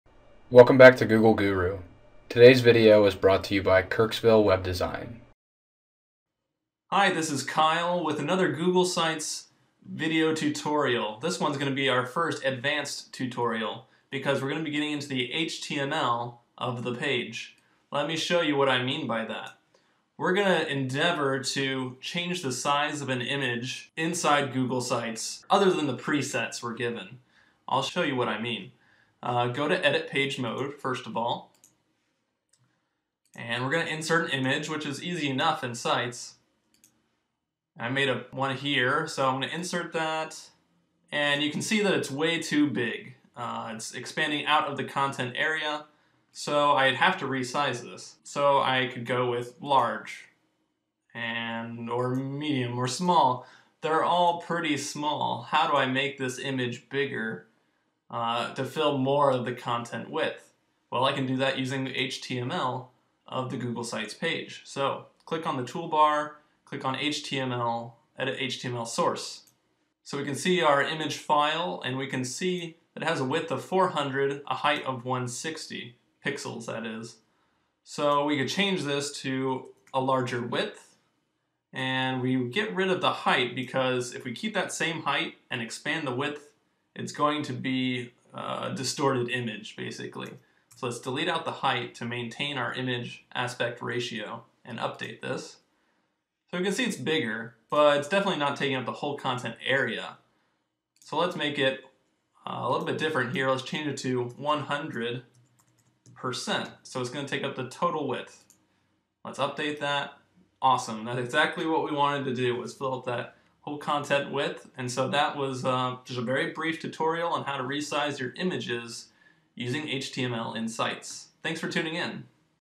Sizing Images in Google Sites with HTML
Here's a quick video about sizing images in Google Sites. Images come with preset sizes like small, medium and large. But sometimes these images don't fit your site like you want. Watch this video to learn how to customize the size of your image using HTML.